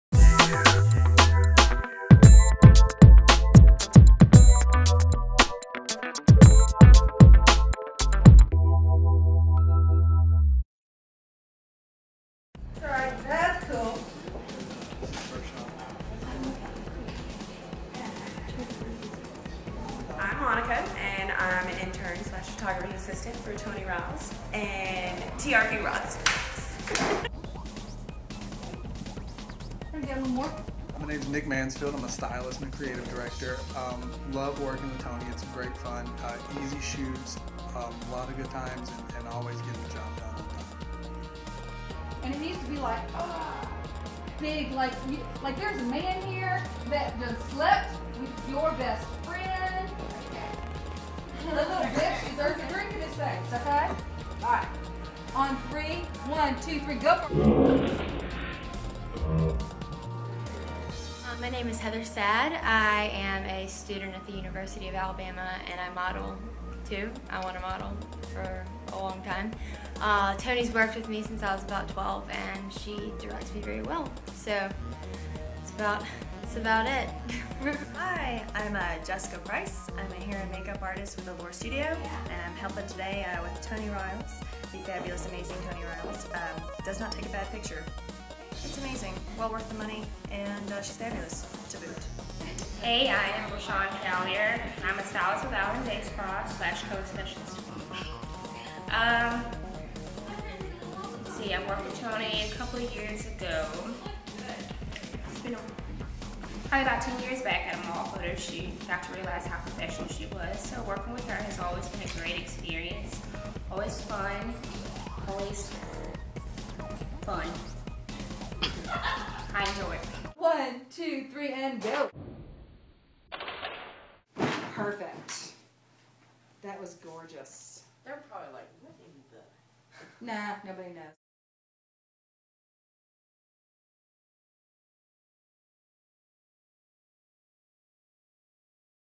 Femme Fatale Shoot
Magazine editorial shoot with the editorial team of Toni Riales Photography